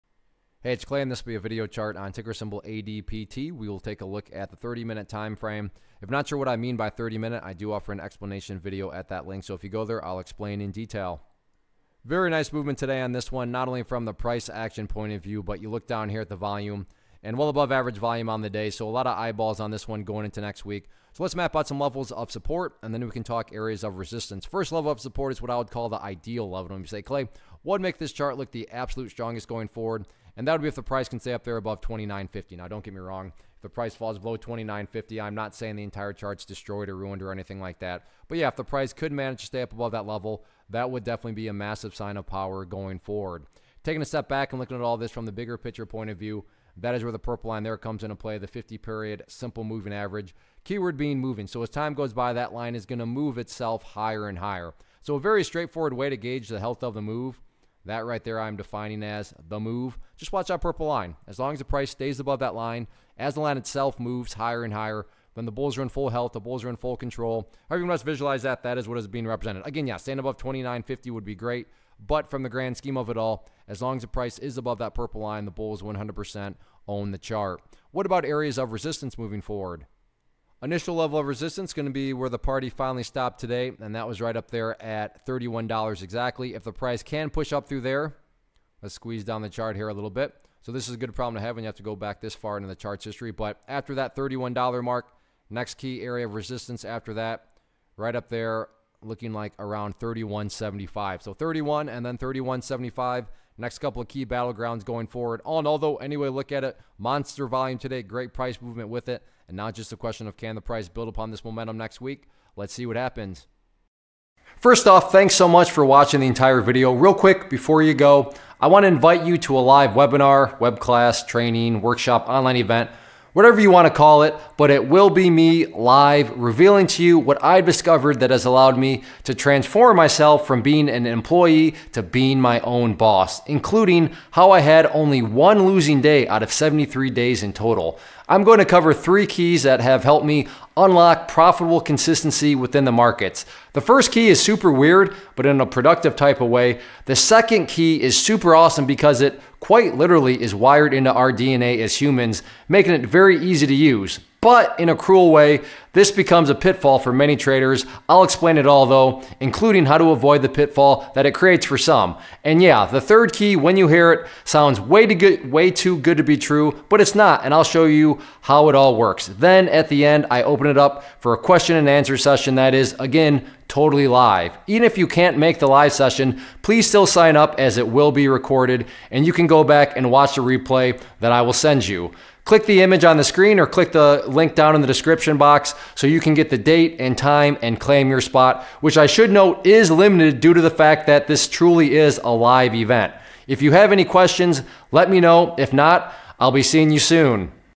Adaptive Biotechnologies Corporation - ADPT Stock Chart Technical Analysis for 01-24-2020
Learn how to read stock charts and identify technical patterns as ClayTrader does a quick stock chart review on Adaptive Biotechnologies Corporation (ADPT).